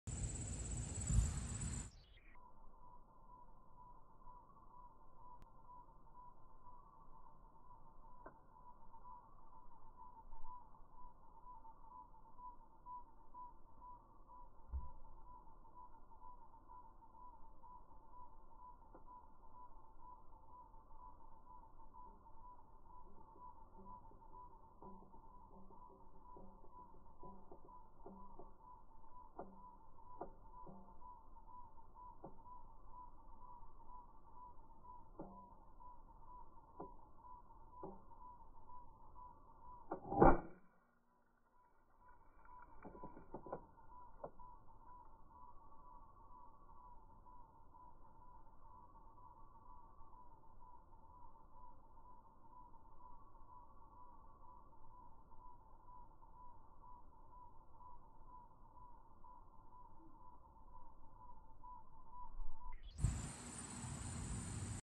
Got em on the fence 7 : Spotted Lantern Fly
Short slow motion video killing a spotted lantern fly using a Bug-A-Salt salt gun.
If you have any ideas on new ways you'd like to see them destroyed, let us know!

For those who need knowing... spotted lantern flies are an incredibly destructive invasive species that even the USDA has advised to be destroyed upon sight. So, there's that.